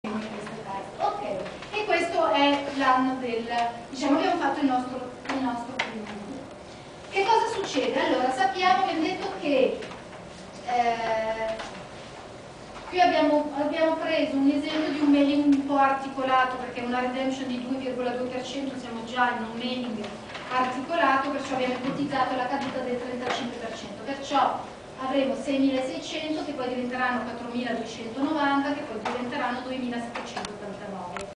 Una sessione del Festival 2008 sul direct mail
Marie Amadei amministratore IDMC parla di "Long term value oppure ROI? La costruzione di un piano triennale
di direct mailing di successo" al Festival del Fundraising 2008